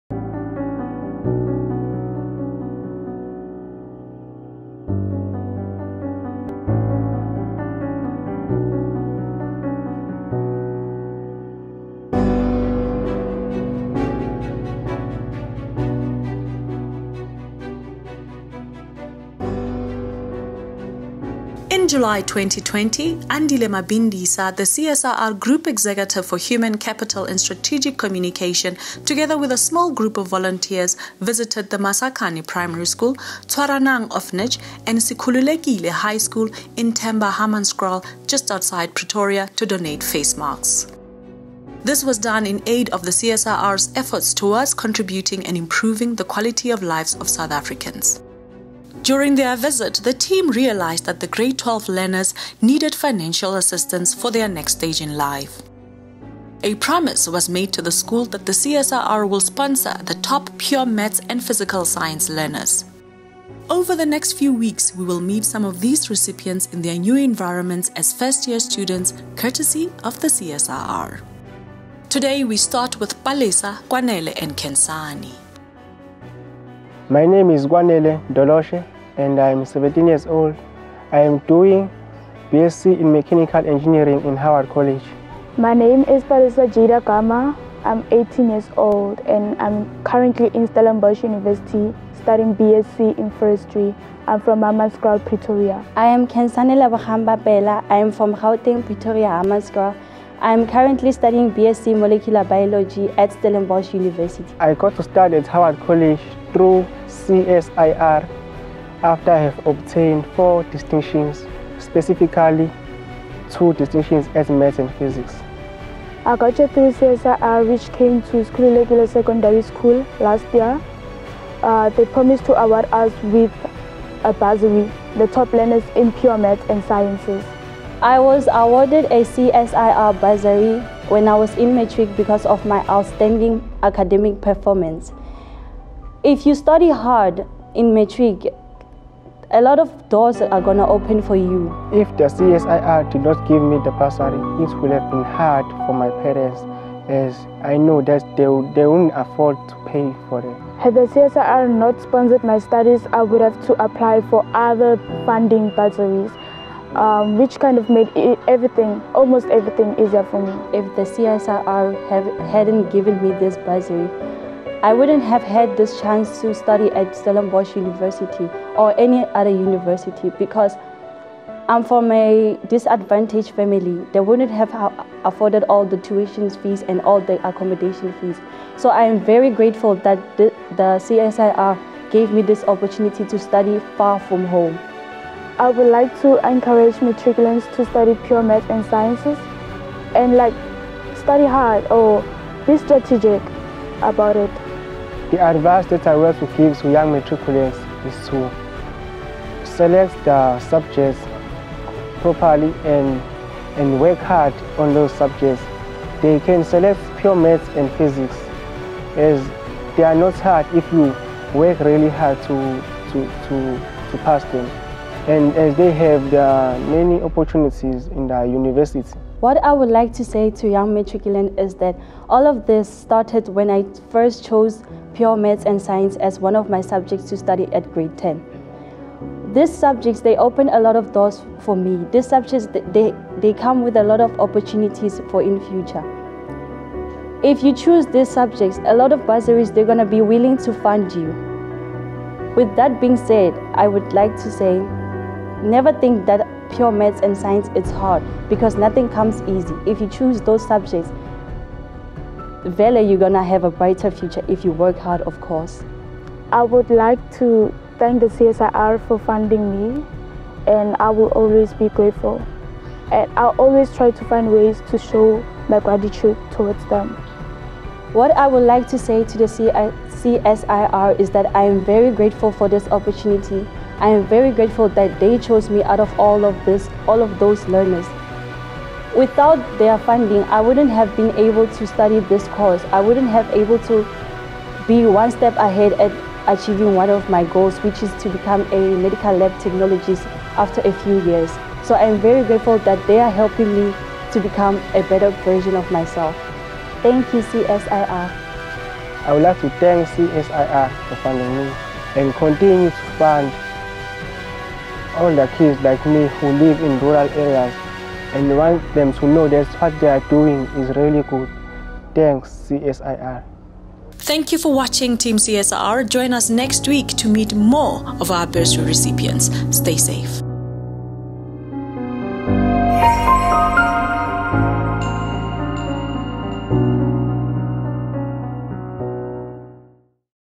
Meet 3 first-year students who are beneficiaries of the CSIR Corporate Social Investment programme
The CSIR’s CSI programme has a strong focus on Education and Skills Development; Socio-Economic Development; and Employee Volunteerism. The initiatives under Education and Skills Development include bursaries for top-performing underprivileged learners who want to pursue careers in science, engineering and technology.

Meet three TeamCSIR first-year bursary recipients and bright young minds Palesa, Kwanele and Khensani. We can’t wait to see what your future holds!